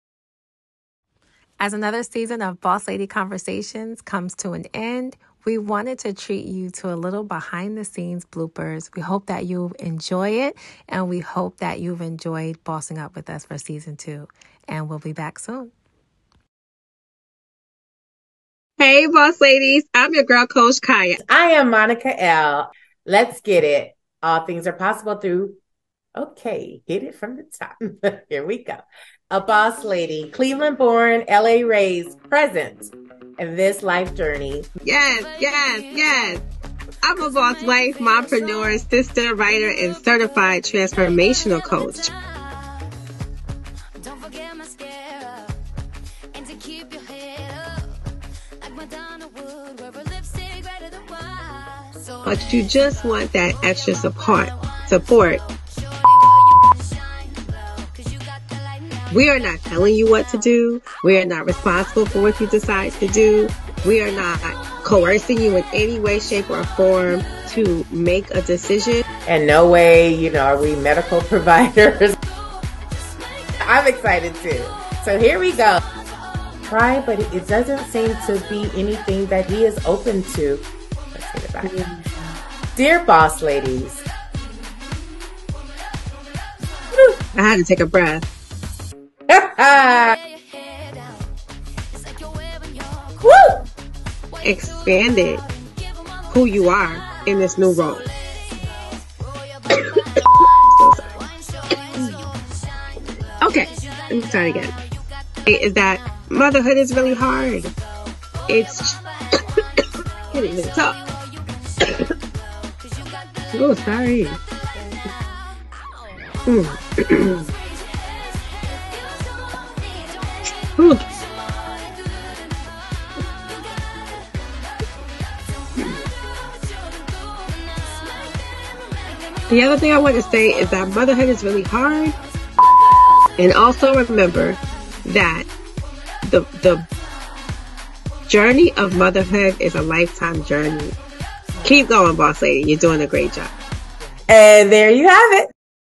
Boss Lady Conversations: Season Two Behind-the-Scenes Bloopers
Thank you for Bossing Up with us all season long! We wanted to end this season on a funny note and share some behind-the-scenes bloopers with you. We make it look easy, but some days Bossing UP is hard work! 😂🥰🥰 Special thanks to our talented brother, @h_u_e_y.s_c_o_t_t for pulling the funny right out with his editing genius. Don’t worry; we’re not going anywhere!  Thank you for your support and 💖!